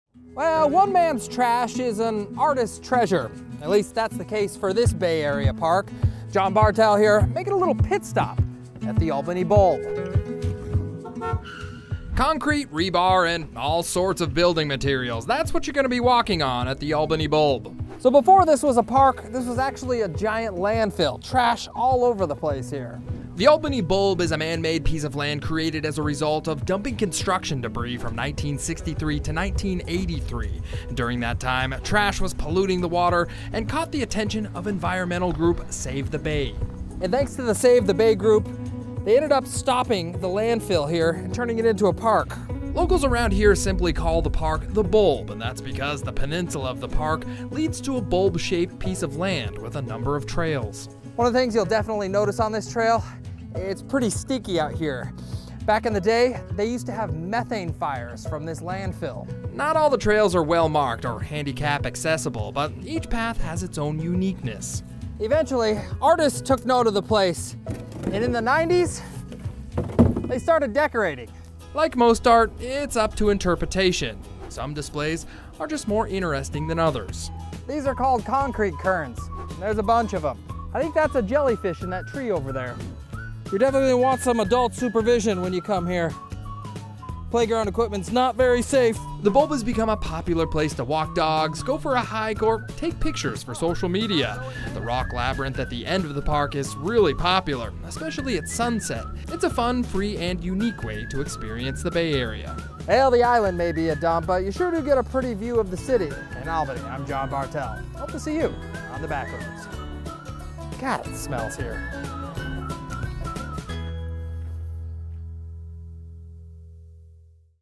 Garbage Art | Bartell's Backroads
The Albany Bulb was a landfill where trash was dumped into San Francisco Bay. Now, it's an art park and a place to catch a beautiful sunset glimpse of the city skyline.